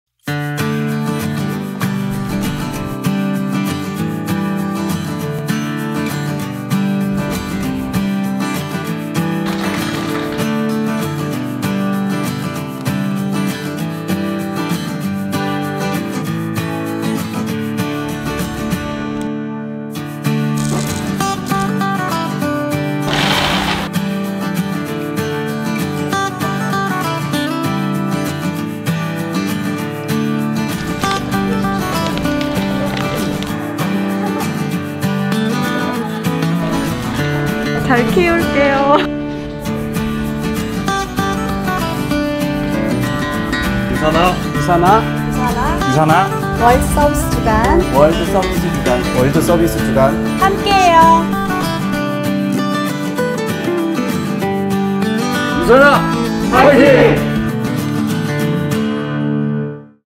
2021 유사나 월드 서비스 주간 - 지구사랑 캠페인
매년 전 세계 유사나 가족들이
따뜻한 나눔을 실천하는
유사나 월드 서비스 주간!

유사나코리아에서는
유사나 제품의 플라스틱과 파우치 용기로
예쁜 미니 화분을 만들며
‘제로 웨이스트’ 캠페인을 진행했는데요🪴

유사나코리아 직원들의
따뜻한 손길로 지구 사랑을 실천한
특별한 현장을 공개합니다 🌍🥰

World Service Week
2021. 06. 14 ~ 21

여러분도 함께해주세요!